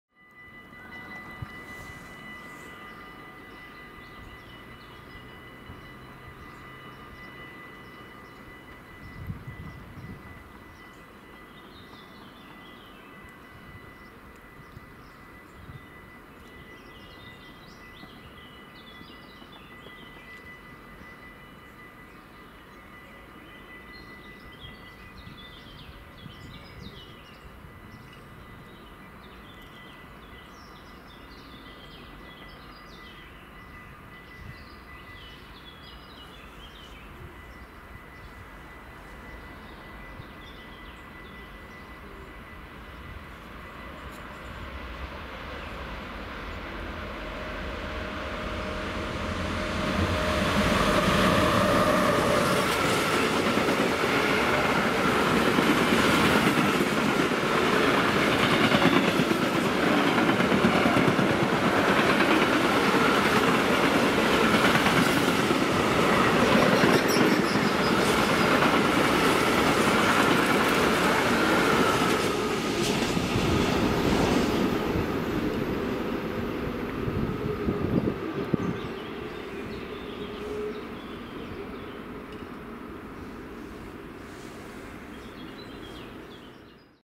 DB Cargo Vectron met mooie lange staaltrein door Helmond 't Hout!
De Betuweroute was dicht, dus rijden de meeste treinen via Venlo. Hier komt een Vectron met een mooie lange staaltrein langs Helmond 't Hout!

► Social Media!